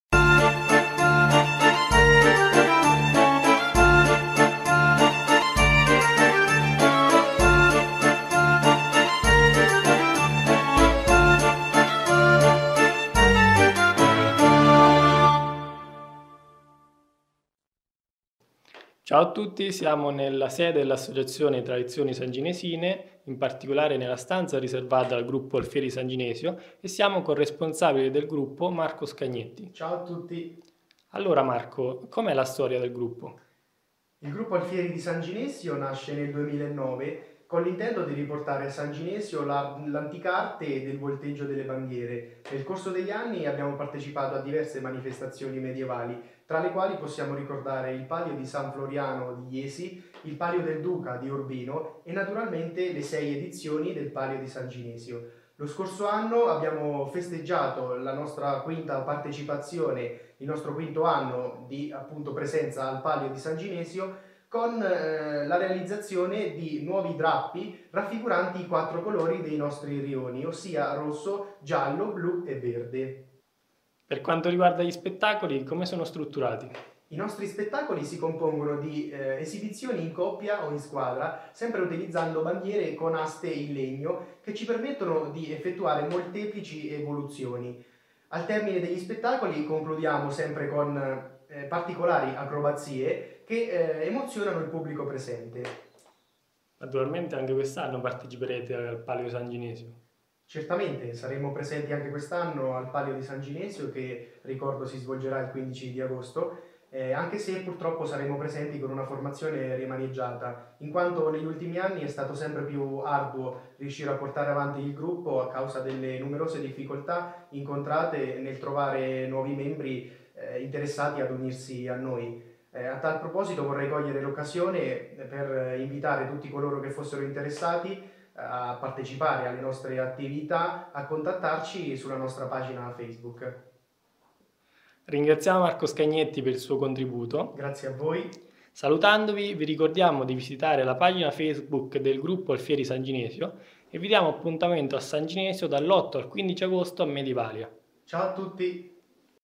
Le interviste di Medievalia 2015 - Gruppo Alfieri San Ginesio
Puntata del Le interviste di Medievalia 2015 con Marco Scagnetti, responsabile del Gruppo Alfieri di San Ginesio.